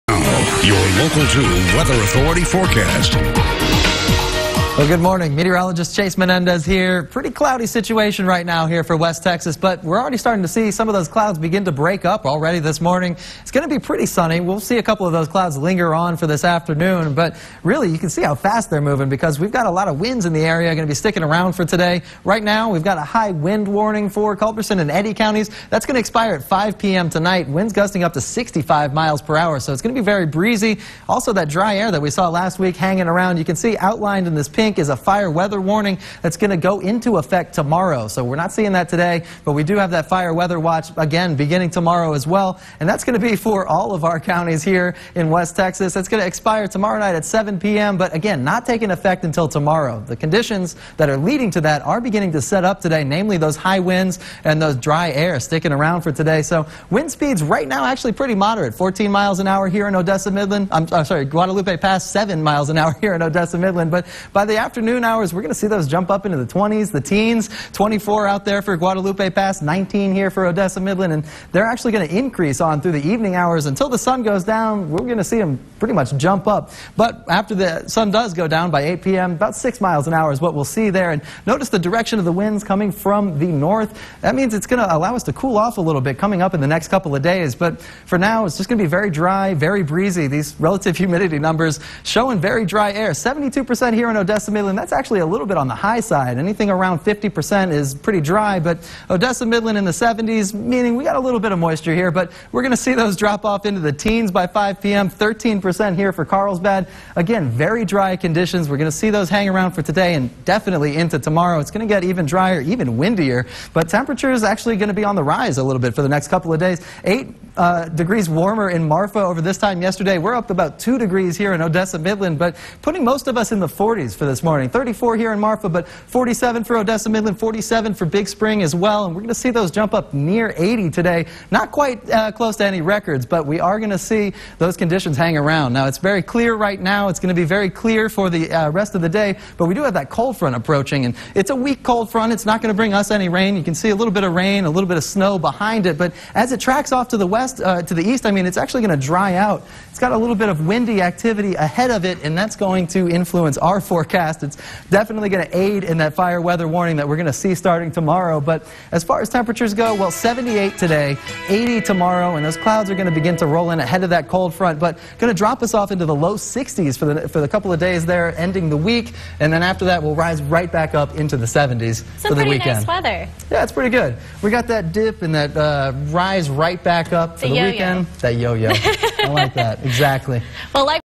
More Fire Weather Ahead - 2/27/2017
Fire Weather Watches already issued for tomorrow, find out what to expect.